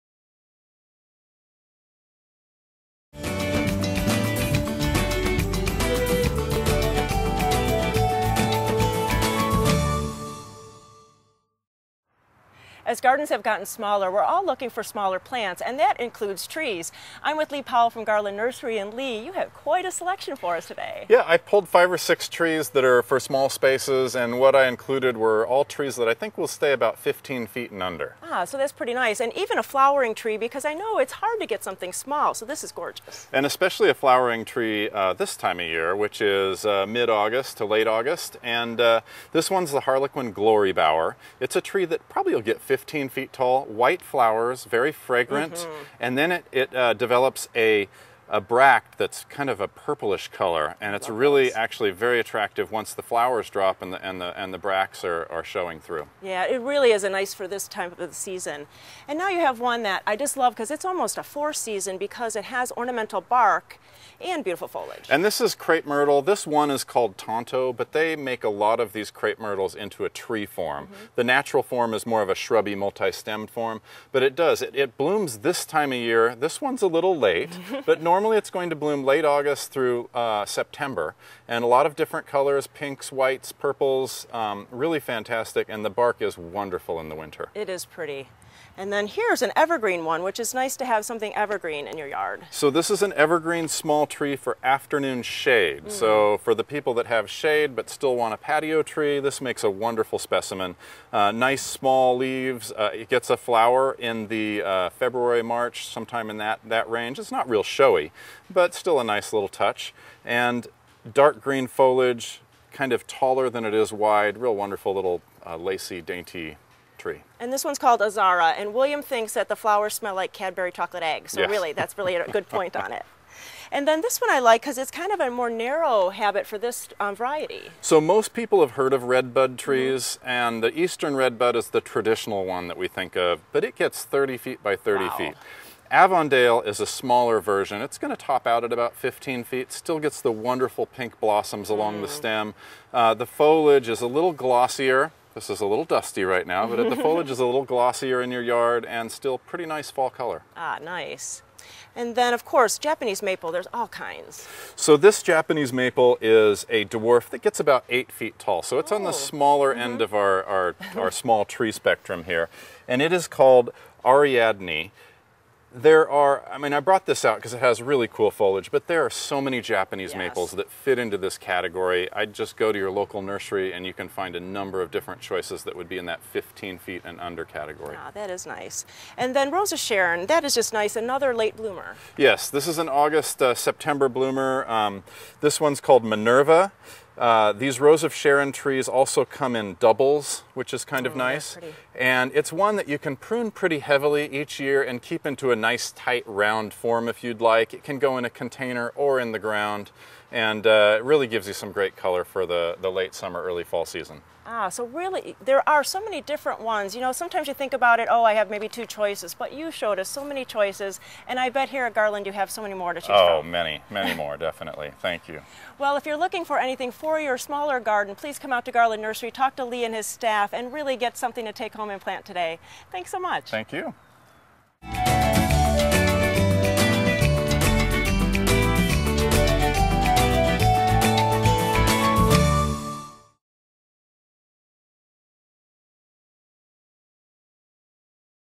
Small Space Trees
If you have a small garden you can still have a tree in it.  we have a few selections to choose from.